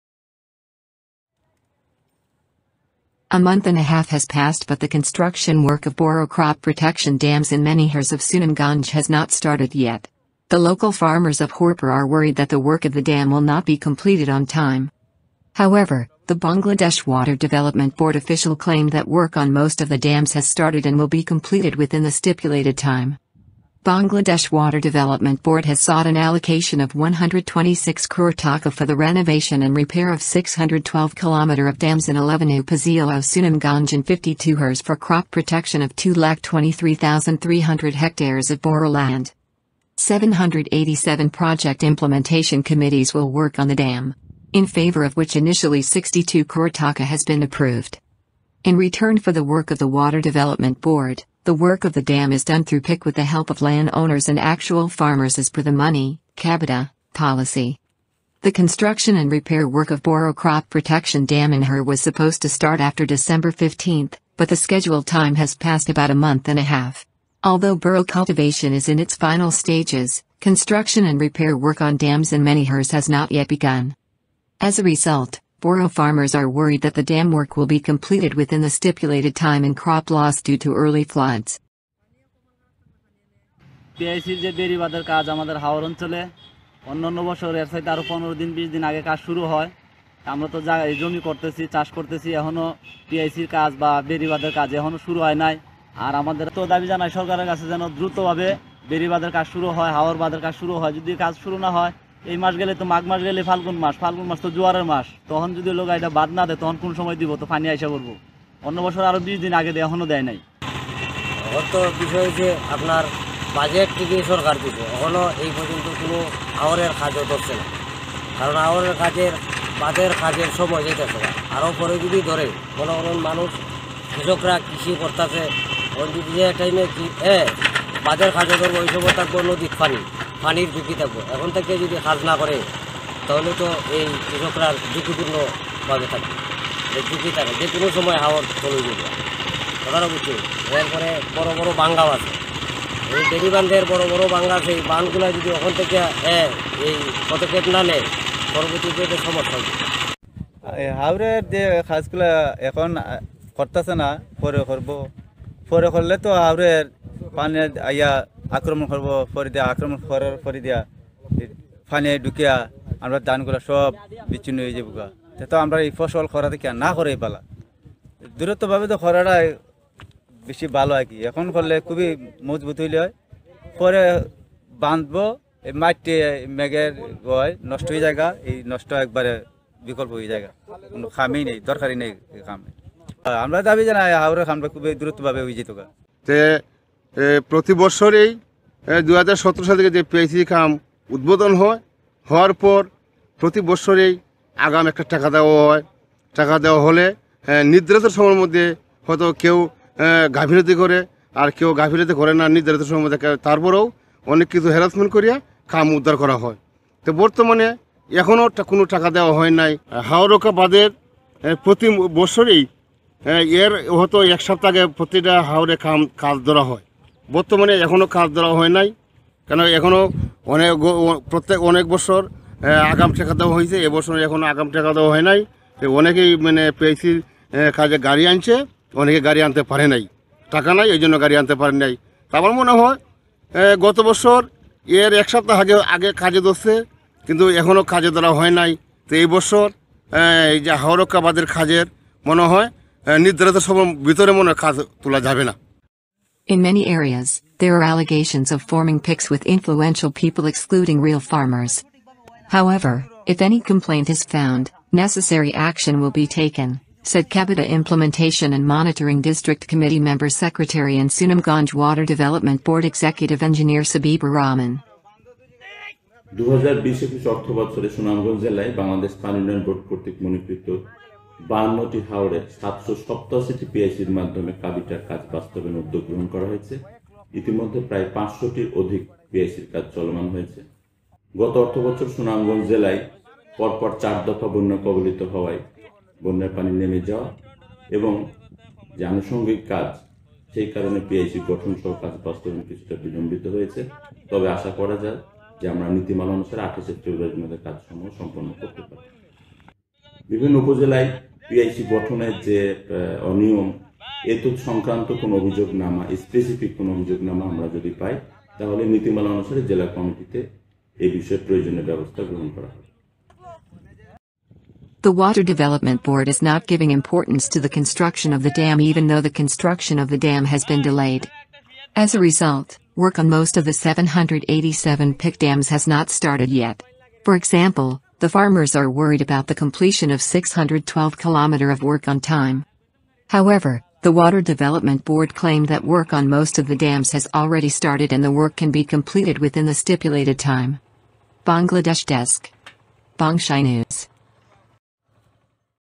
Although A Month And A Half Has Passed, Work Of Crop Protection Dam Has Not Started In Haor
A month and a half has passed but the construction work of boro crop protection dams in many haors of Sunamganj has not started yet. The local farmers of Haorpar are worried that the work of the dam will not be completed on time. .
However, the Bangladesh Water Development Board official claimed that work on most of the dams has started and will be completed within the stipulated time.

Bangladesh Water Development Board  has sought an allocation of 126 crore taka for the renovation and repair of 612 kilometer of dams in 11 Upazila of Sunamganj in 52 haors for crop protection of 2 lac 23 thousand 3 hundred hectares of boro land.

787 Project Implementation Committees will work on the dam. In favor of which initially 62 crore taka has been approved.

In return for the work of the Water Development Board, the work of the dam is done through PIC with the help of land owners and actual farmers as per the money (kabita) policy.

The construction and repair work of Boro Crop Protection Dam in Haor was supposed to start after December 15, but the scheduled time has passed about a month and a half. Although borough cultivation is in its final stages, construction and repair work on dams in many haors has not yet begun.

As a result, boro farmers are worried that the dam work will be completed within the stipulated time and crop loss due to early floods.

In many areas, there are allegations of forming PICs with influential people excluding real farmers.

However, if any complaint is found, necessary action will be taken, said Kabita Implementation and Monitoring District Committee Member Secretary and Sunamganj Water Development Board Executive Engineer Sabibur Rahman.

The Water Development Board is not giving importance to the construction of the dam even though the construction of the dam has been delayed.

As a result, work on most of the 787 PIC dams has not started yet. For example, the farmers are worried about the completion of 612 kilometer of work on time.

However, the Water Development Board claimed that work on most of the dams has already started and the work can be completed within the stipulated time.

Bangladesh Desk
Bongshai News.